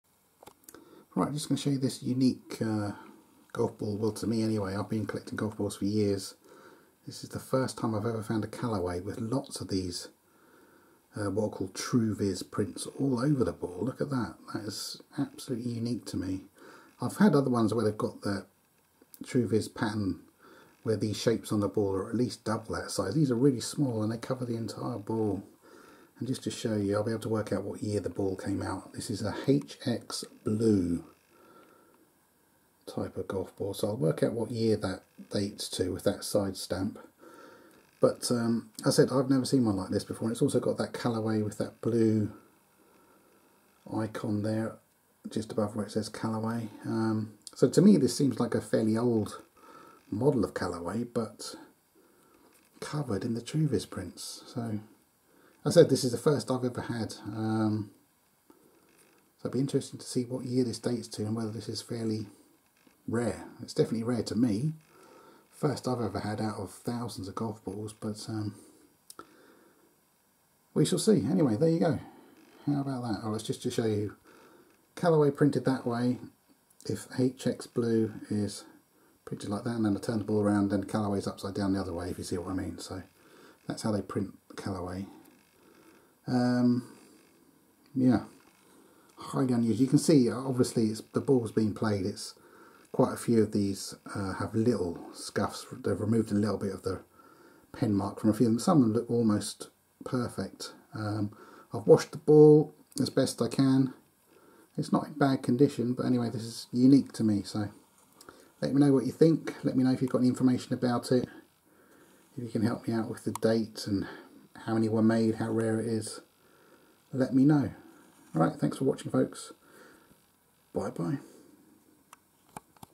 Rare Callaway HX Blue Truvis Golf Ball 26sep23 Cambs UK
first i have ever seen like this, this type of ball dates to approx 2001 i think